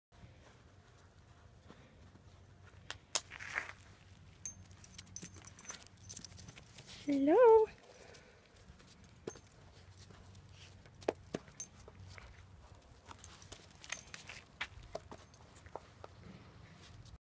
Meet Lizzy Caplan!
Meet Lizzy Caplan, an 8-month-old Rat Terrier, who loves her toy duck!